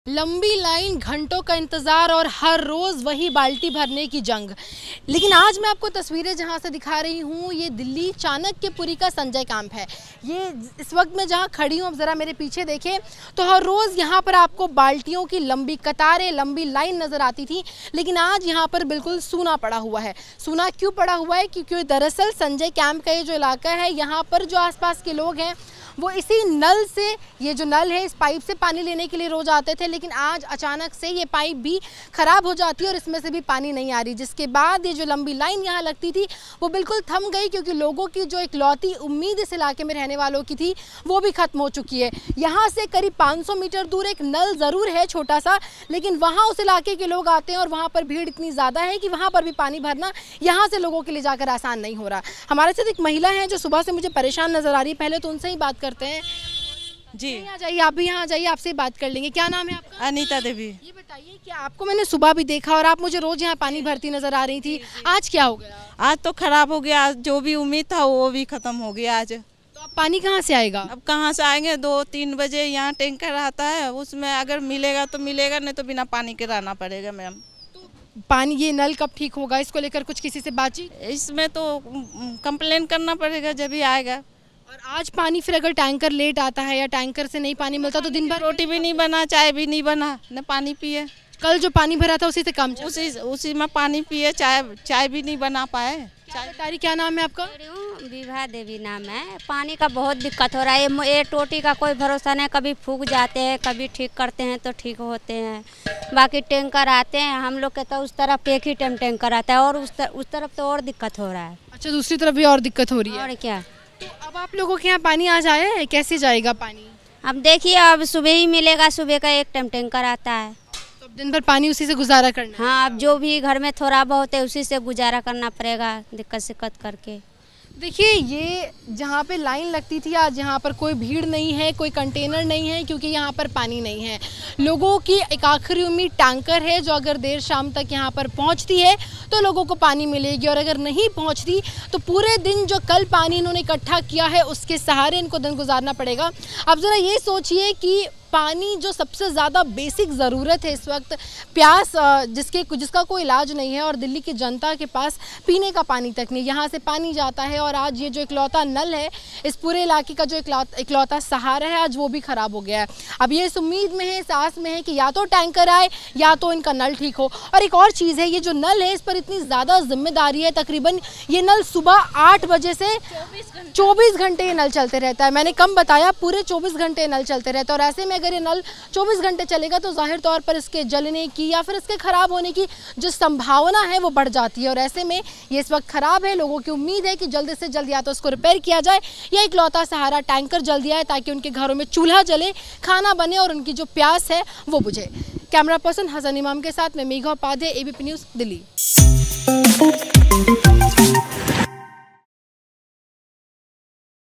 Delhi Water Crisis : राजधानी दिल्ली में पानी हुआ खत्म? लोगों को सुनकर रह जाएंगे हैरान!
ABP News: चाणाक्यपुरी के संजय कैम्प के जिस नल के आगे बालटियों का तांता लगा होता है आज सब वीरान है। न लाइन न लड़ाई न लोग। इस इलाक़ें में रहने वालों लोगों की उम्मीद वो नल भी आज ख़राब हो गया है। सुबह लोग पानी भरने आए तो थे लेकिन बिना पानी लिये उदास चेहरों के साथ लौट गये । अब लोग आस लगाये नल के ठीक होने या टैंकर के आने का इंतज़ार कर रहे हैं। कल जिसके घर जितना पानी था आज भी उतने से काम चलाना होगा।